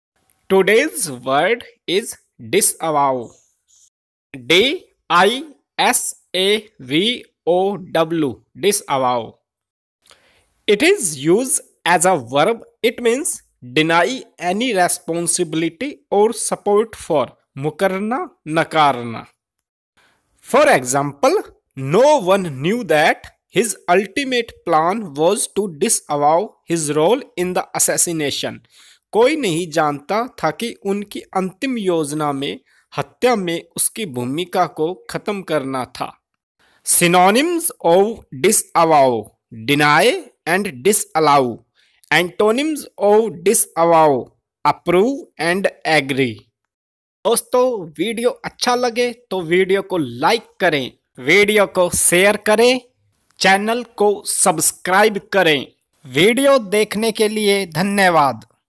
Disavow meaning
Disavow
डिस‌अवाओ

verb

deny any responsibility or support for
मुकरना, नकारना
No one knew that his ultimate plan was to disavow his role in the assassination.
कोई नहीं जानता था कि उनकी अंतिम योजना में हत्या में उसकी भूमिका को खत्म करना था।
Synonyms:- 1. Deny
                     2. Disallow
Antonyms:- 1. Approve
                    2. Agree